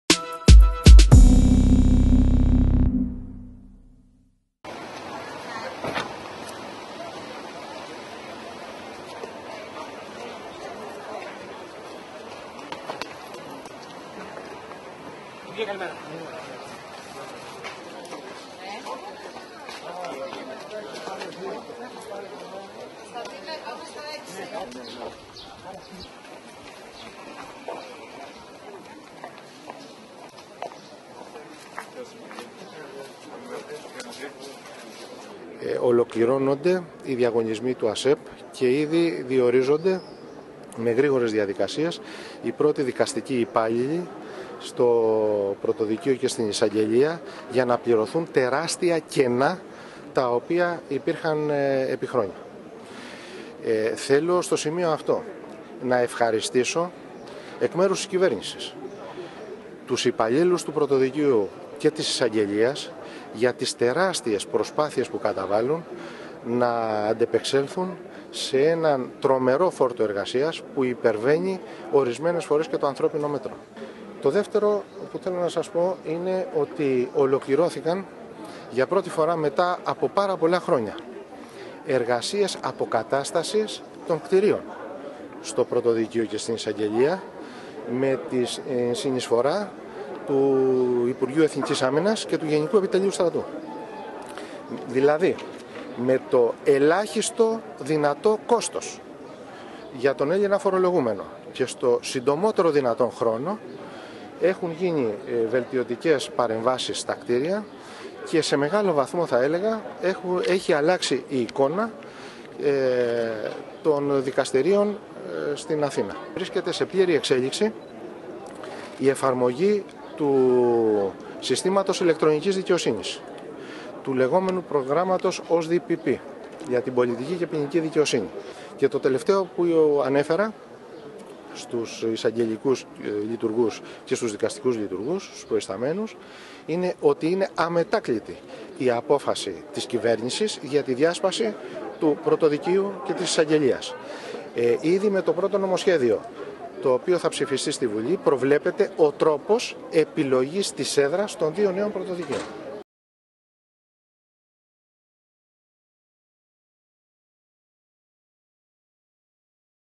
Στην Εισαγγελία Πρωτοδικών ο Σ. Κοντονής
Ο Υπουργός Δικαιοσύνης, Διαφάνειας και Ανθρωπίνων Δικαιωμάτων Σταύρος Κοντονής επισκέφθηκε την Εισαγγελία Πρωτοδικών και το Πρωτοδικείο Αθηνών.απε-μπε/Α.Ρεμούνδος